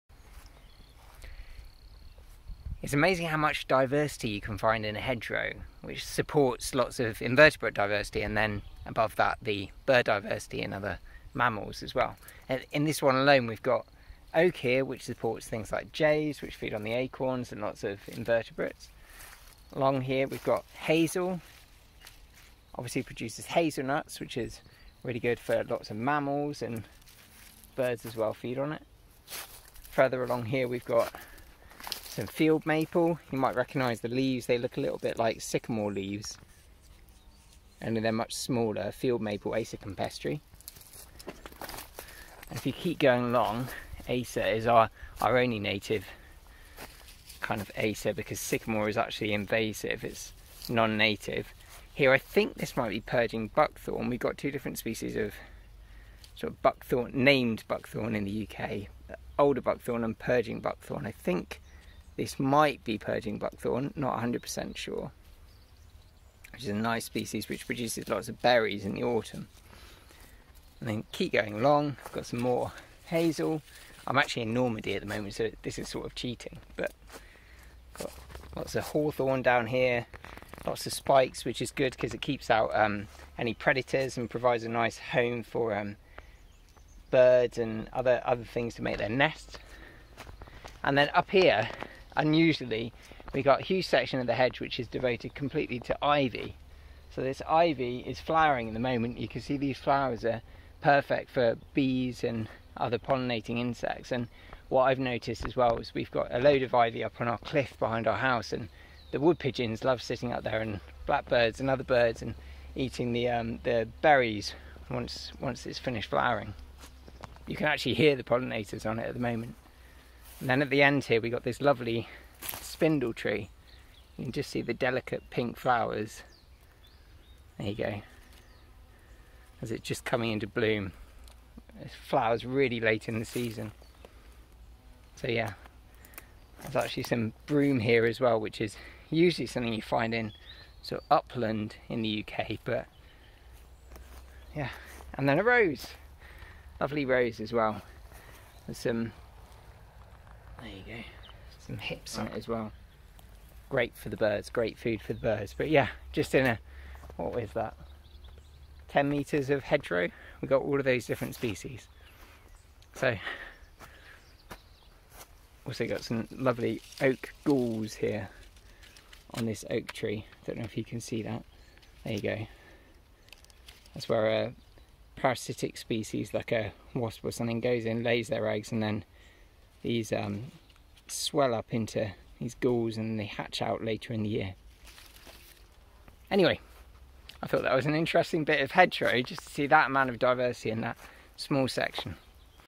Find Local Native Trees & Shrubs - How to Rewild
After giving a talk on rewilding recently, I was approached by a garden designer in the audience. "You talked about planting suitable native species, but is there somewhere I can find information about native species to plant?".

Well, there are places which provide this type of general information - The Woodland Trust in particular, has a great guide called 'Twigged' and Wikipedia is a legitimately useful tool for plant information - but I find that the best place to find trees and shrubs which are suitable for YOUR area is by looking for a really diverse hedgerow like this one. There are probably a few near your patch (also check in local nature reserves and on SSSIs), and they'll be a testbed for the types of species which will thrive on your land.

Have a problem with deer? Then you probably won't have a huge amount of Hazel in your hedges - they'll tend towards thornier species and those with tall trunks which grow up through the protection of the thickets.

If you've got a waterlogged soil, then you'll likely see more Alder, Willow and Downy Birch springing up. In contrast, drier places will get more Gorse and perhaps some Scots Pine.  There are many other factors to consider, from soil acidity to soil type, sun and wind exposure and hardiness zone, but that's why a hyper-local hedge is best!

You can probably find a decent enough Plant ID app on your phone (Google Lens is great and built into the camera on many Android devices), which will help you recognise what you're looking at. Bear in mind that you'll see introduced and invasive species in there, too, so it's worth doing a little bit of reading, to check that you're working with native species.

Remember that, even if it's growing wild in a hedge, it may be perfectly happy as a tree (e.g. Holly, Hornbeam, Hawthorn), so feel free to give the plants some room. And collecting local seed is a great way of retaining the genetic diversity and 'ecotype' which has evolved to your specific environmental conditions.

Good luck, and happy hedge hunting!